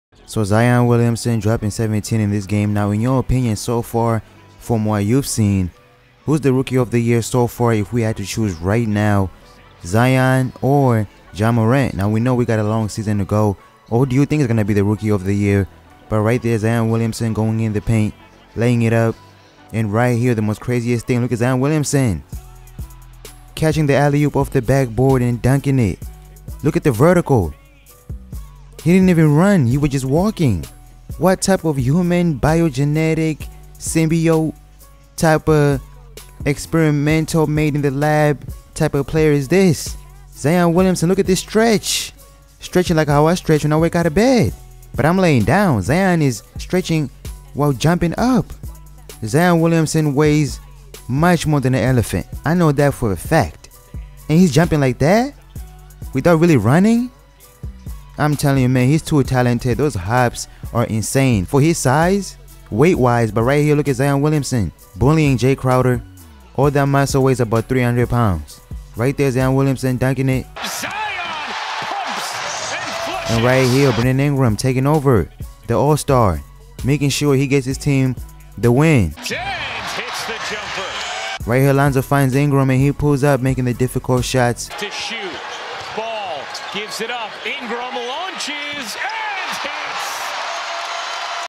Zion Williamson Crazy Backboard Alley-Oop Dunk & Brandon Ingram Takes Over! Pelicans vs Heat
Zion Williamson Crazy Backboard Alley-Oop Dunk & Brandon Ingram Takes Over! Pelicans vs Heat March 6, 2019-20 NBA Season